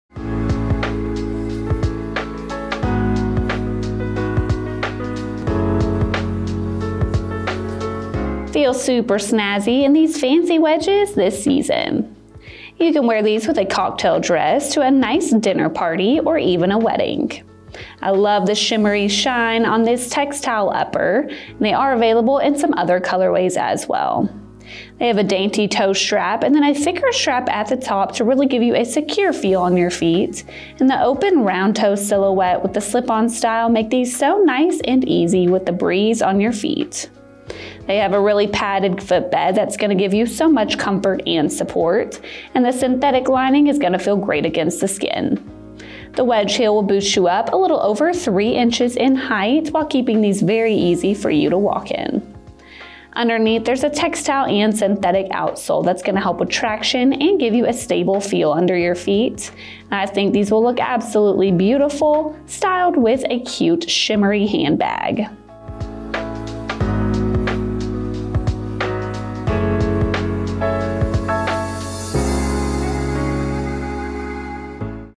CL By Laundry Fanciful SKU: 9716944
The CL By Laundry&reg; Fanciful Heels keep your feet comfy and your look fashionable.
Textile upper.
Synthetic lining.
Signature brand detailing on the insole.
Slip-on style.
Open toe with square toe design.
Wedge heels.
Textile and synthetic outsole.
Imported.